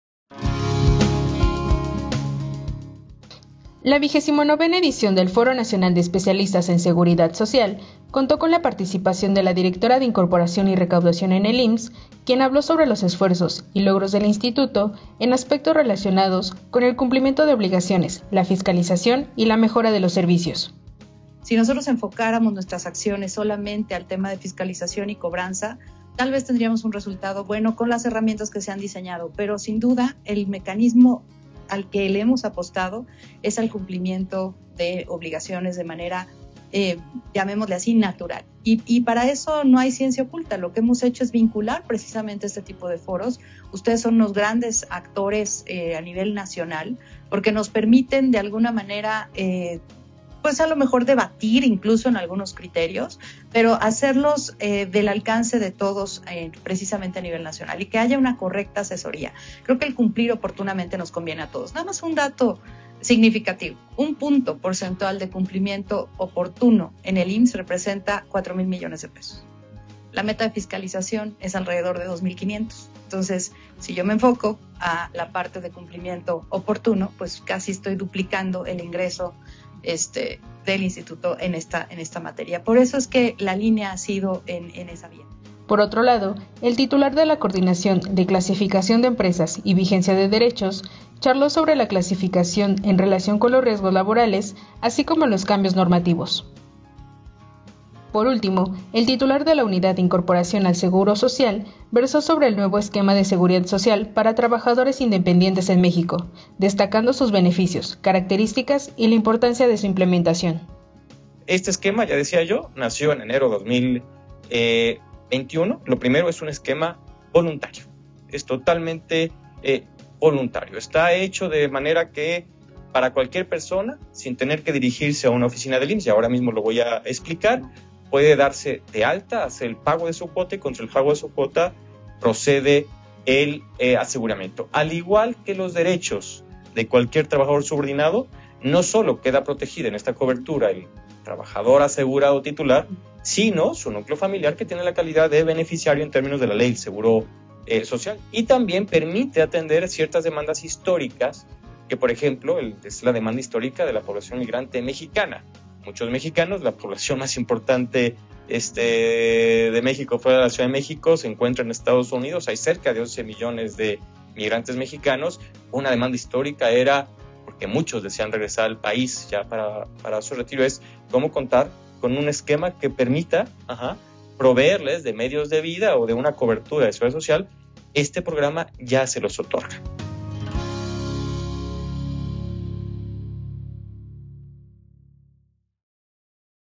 Avances del IMSS en seguridad social
Autoridades del IMSS dialogan sobre los avances y compromisos en SeguridadSocial durante el 29. °FONESS.